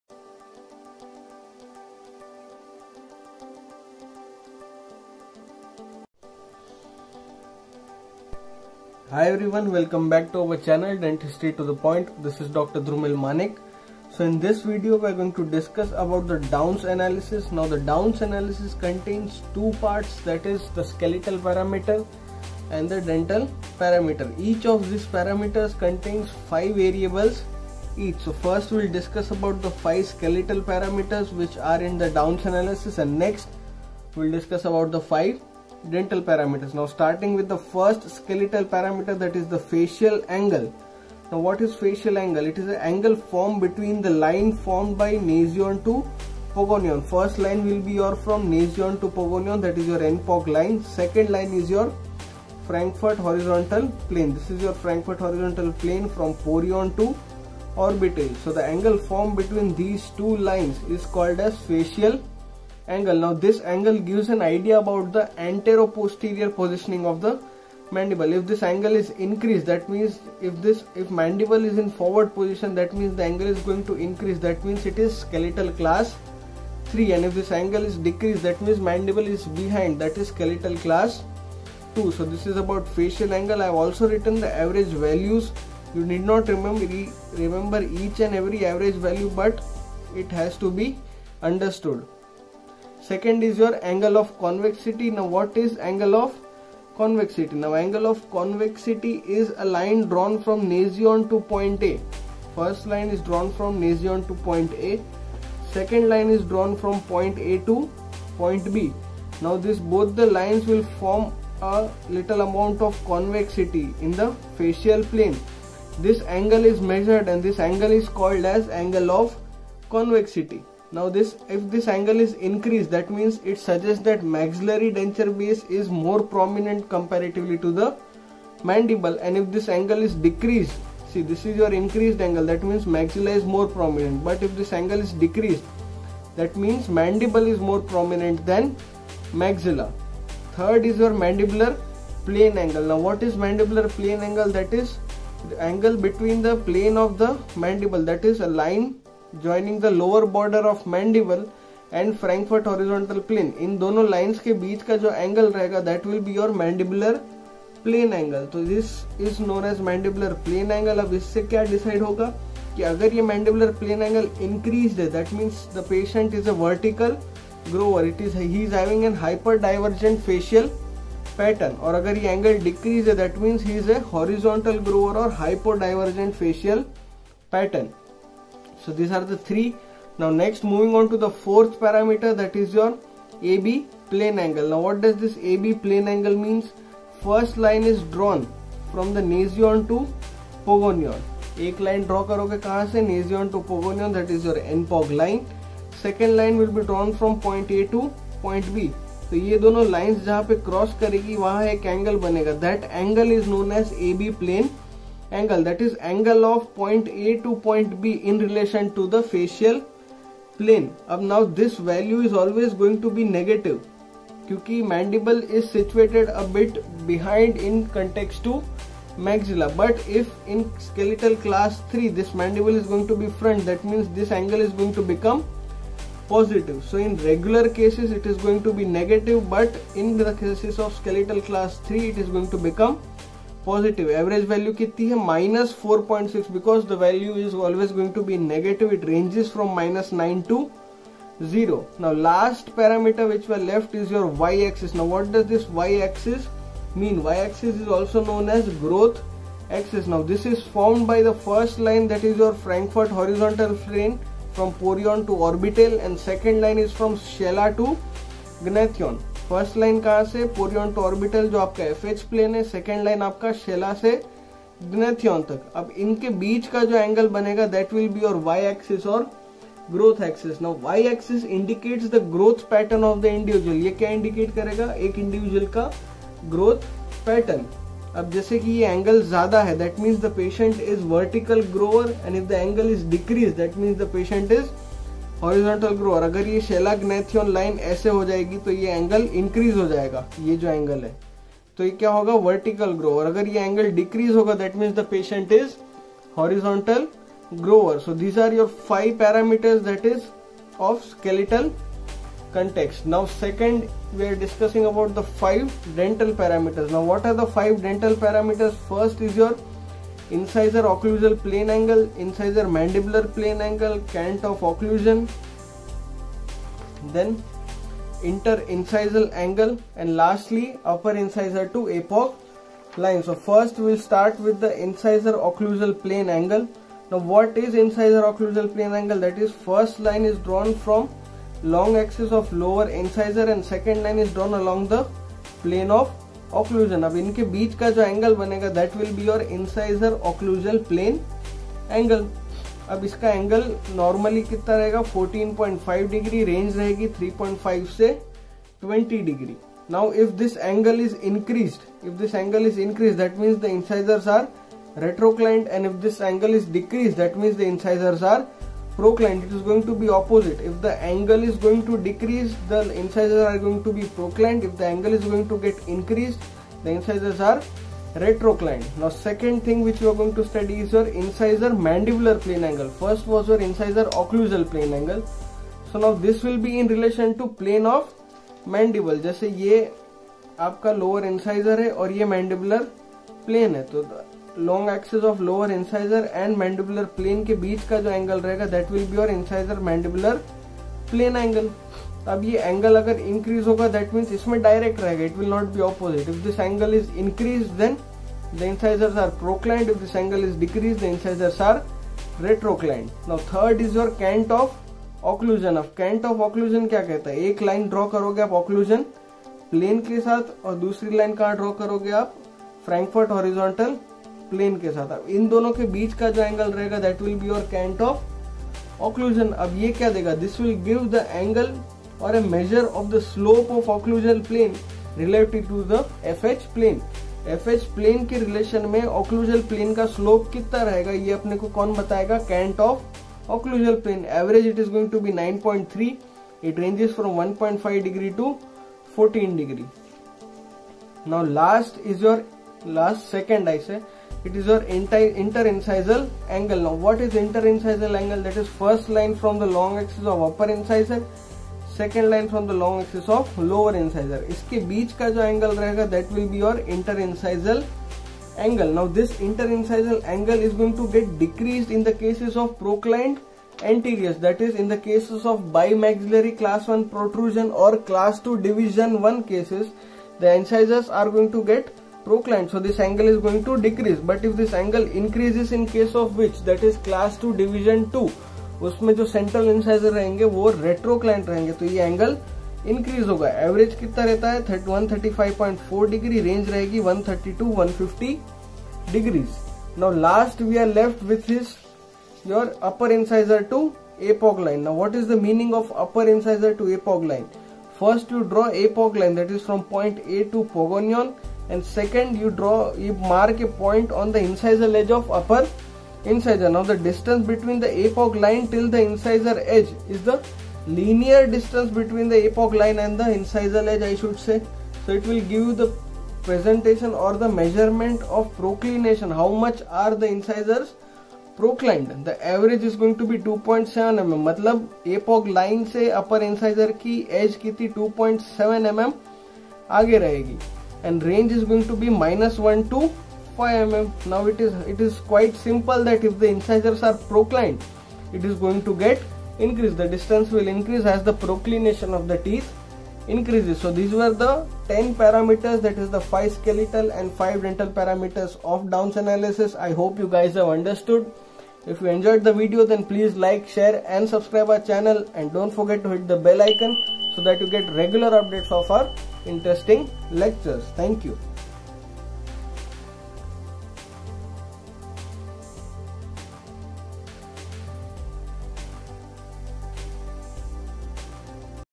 DOWNS ANALYSIS
This video is based on DOWNS ANALYSIS , the various skeletal and dental parameters diacussed briefly for better understanding and long term remembrance.